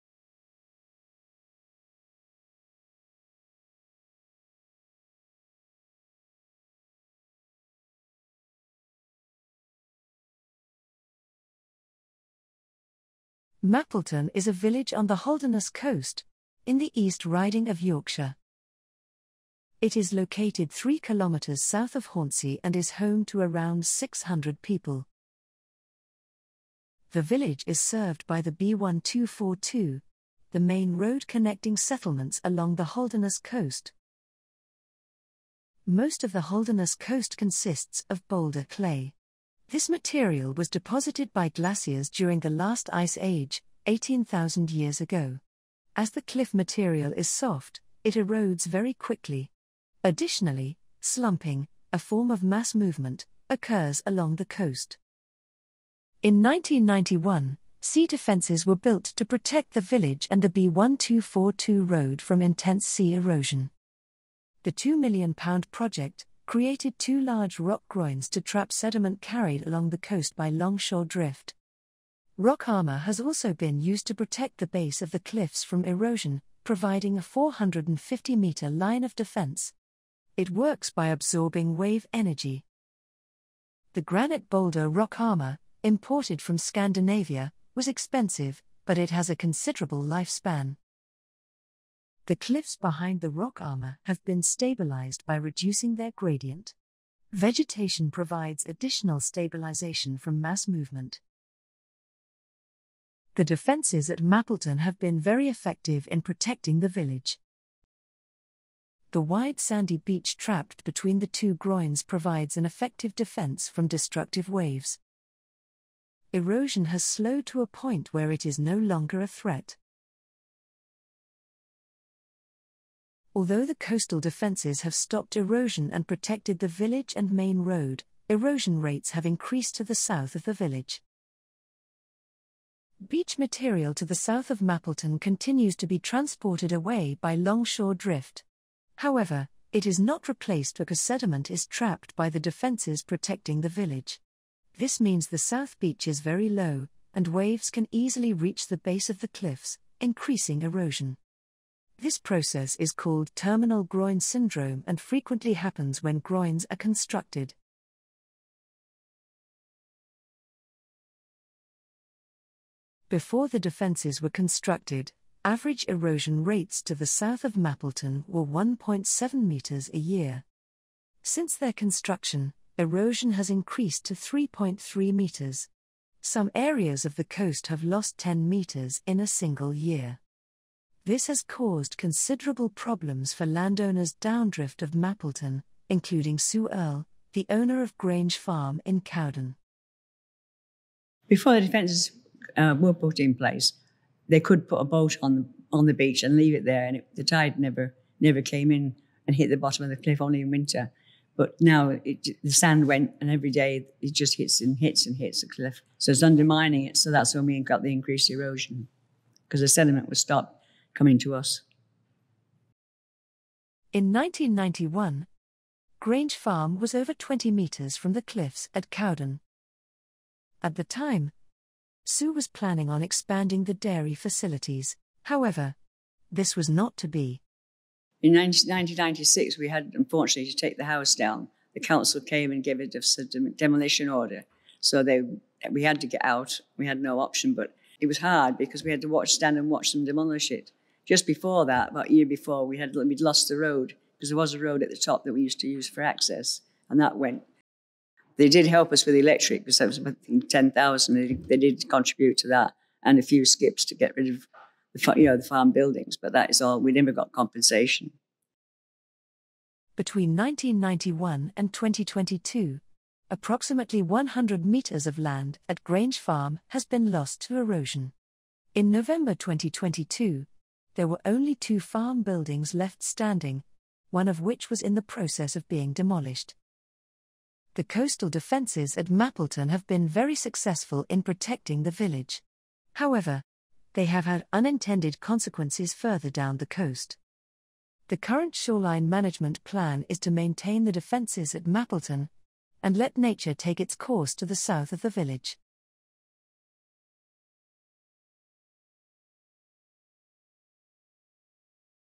Coastal Management at Mappleton
This video explores the impact of coastal management at Mappleton on the Holderness Coast in the East Riding of Yorkshire.